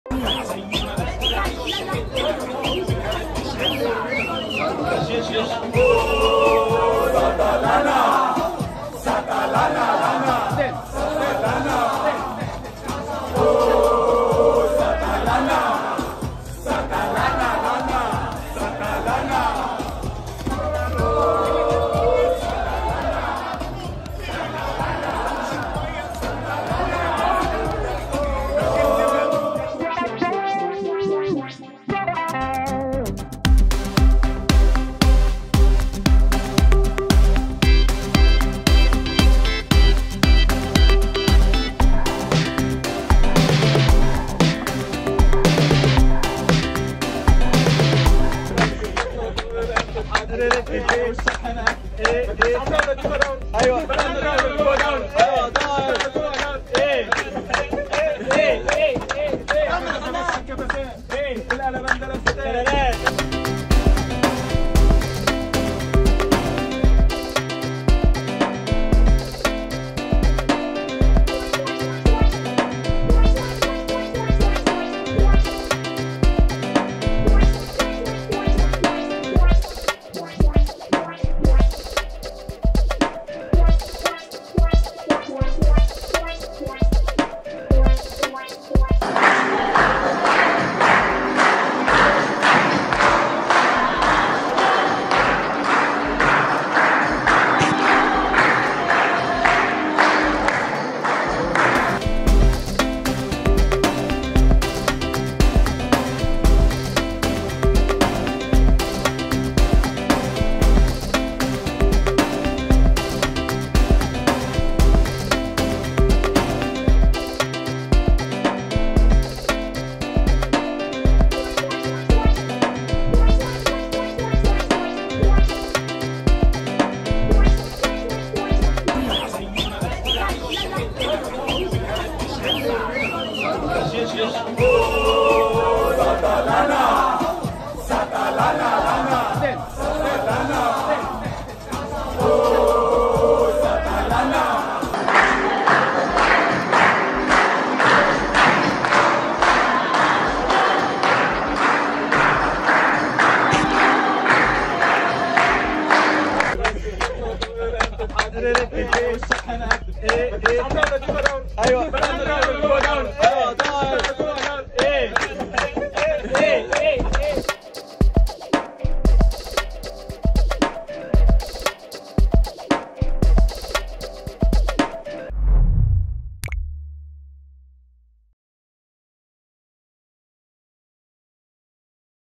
فيديو مجمع باحتفالات طاهر محمد طاهر 🥁🦅👀.. نتحداك ستشاهده أكثر من مرة
أصبحت احتفالات طاهر محمد طاهر لاعب الأهلي علامة مميزة في عقول مشجعي الفريق الأحمر

ا#حتفالات_طاهر